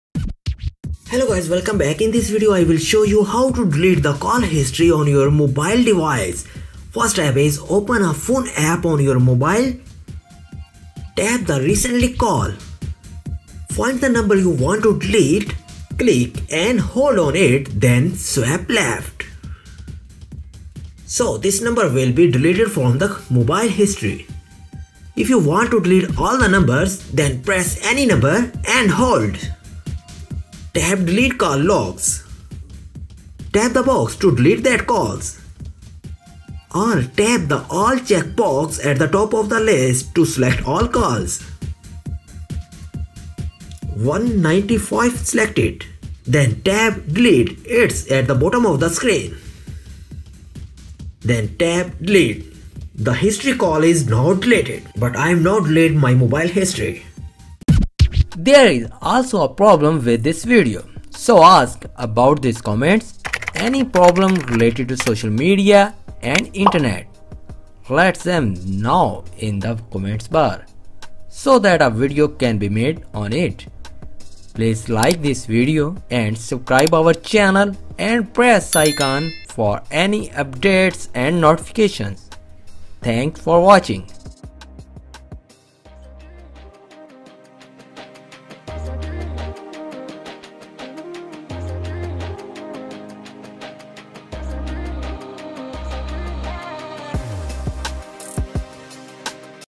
How to Delete Call History on Android Permanently | Do It Yourself.
In This Video I Will Show You How to Delete the Call History on Android.

How to Delete Call History on Android.
Open the Phone app.
Then tap recants call.
Find the number you want to delete and click and hold on it then swap left. So this number will be deleted from the history.
If you want to delete all the numbers.
Then press any number and hold then tap delete call logs.
Tap the box to delete that call, or tap the All checkbox (at the top of the list) to select all calls.
Tap Delete. It's at the Bottom of the screen. The call history is now deleted.